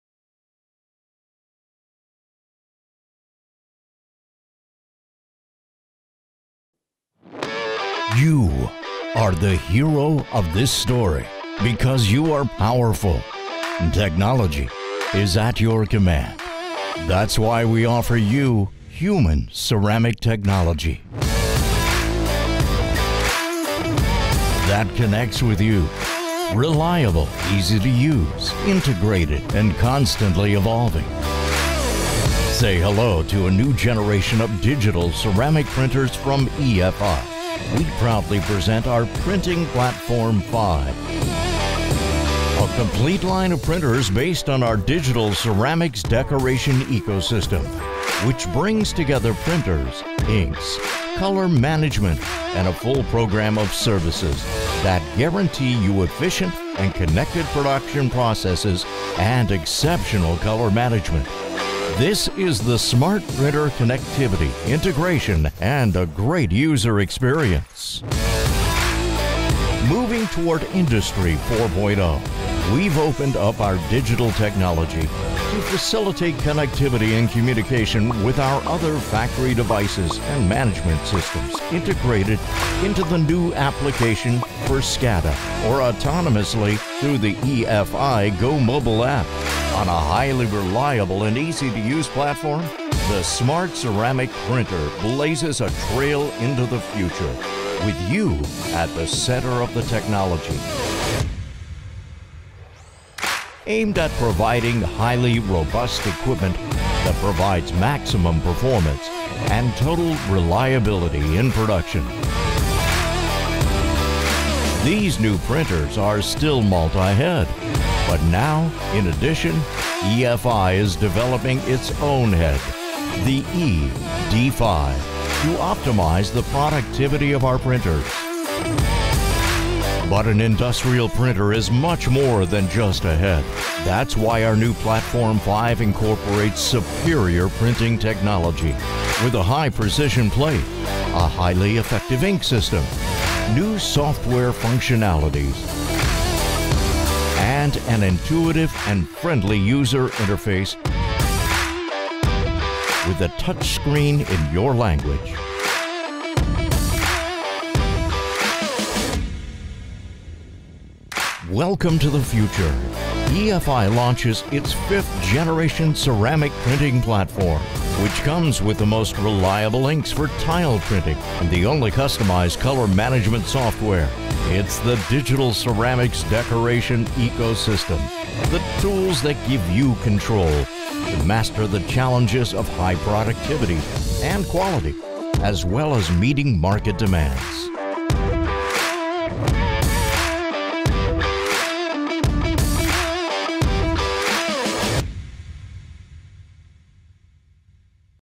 5th Generation of Cretaprint Ceramic Printers
Smart Printer stands for unprecedented functions driving connectivity, software applications, and user experience. A family of printers covering any tile size is available on 5th generation.